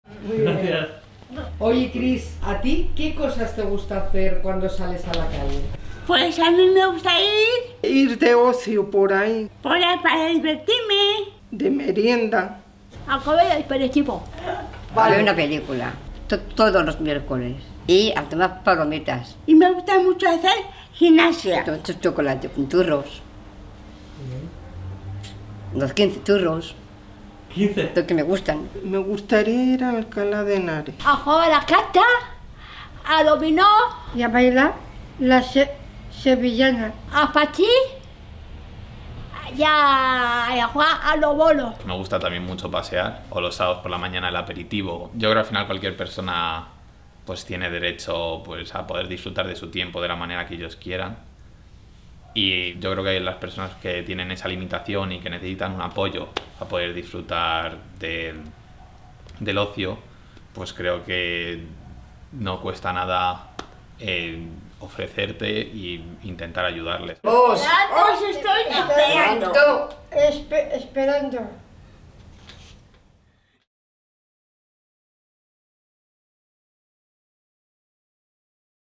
Vivir mi vida
En la fundación buscamos voluntarios para apoyar el desarrollo del ocio de personas mayores con discapacidad intelectual. Elegir lo que hacemos y con quien compartimos nuestro tiempo libre es un derecho necesario para construir una sociedad más inclusiva.